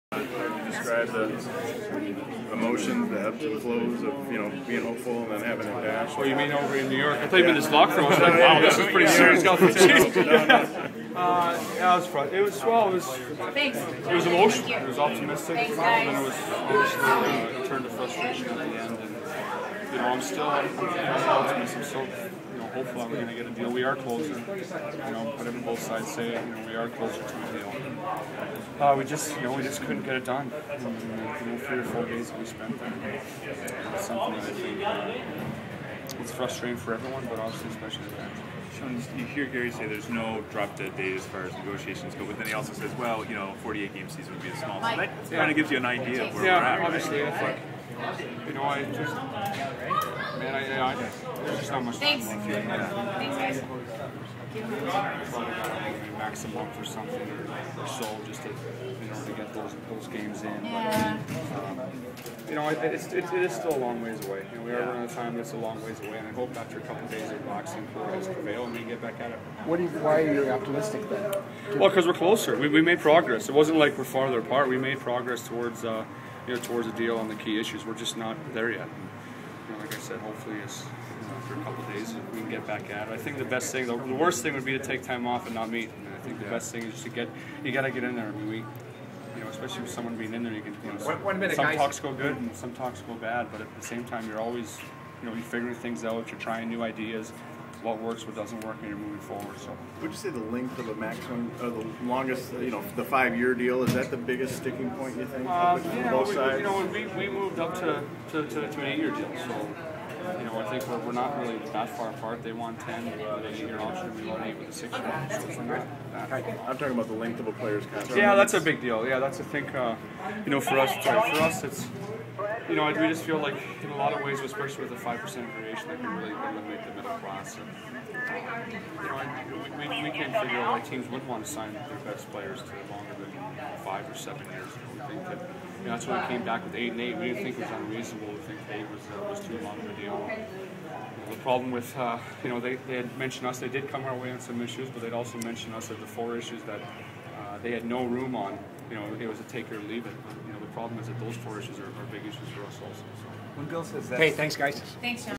Shawn Horcoff
Edmonton Oilers Shawn Horcoff discusses state of NHL CBA negotiations.